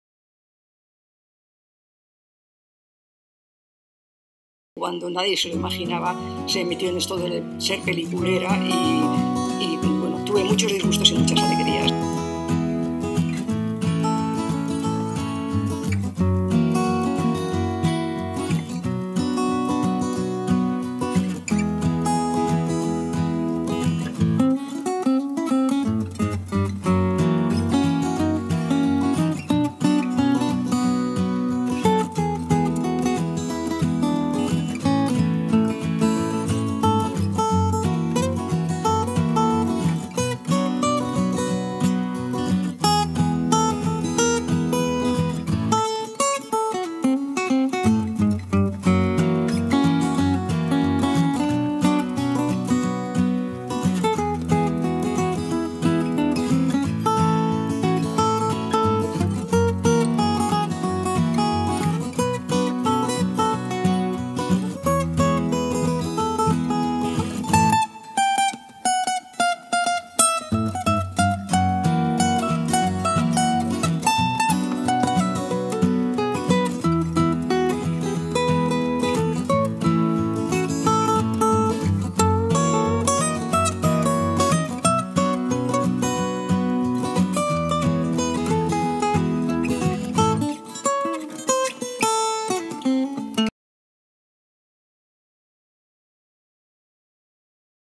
CECILIA BARTOLOME "UNA PELICULERA" La dona mira Cátedra Berlanga CEU UCH
"Cecilia Bartolomé. Una peliculera” es un audiovisual creativo que homenajea a la cineasta Cecilia Bartolomé, una de las primeras mujeres diplomadas en la Escuela de Cine en los años 60, Medalla de Oro al Mérito en las Bellas Artes y premio especial de la Academia Valenciana del Audiovisual en 2018.

Este audiovisual, realizado por dos estudiantes de la Cátedra de Investigación Luis García Berlanga, abrió el programa de actividades de la Jornada “La dona mira. Mira la dona”, organizada por la Corporació Valenciana de Mitjans de Comunicación (CVMC) y À Punt Mèdia, en marzo de 2018, y que contó con la presencia de la propia Cecilia Bartolomé.

REALIZACIÓN AUDIOVISUAL: Sandra Rodrigo Guillamón e Irene Valdés Valiente, con el asesoramiento de Begoña Siles Ojeda.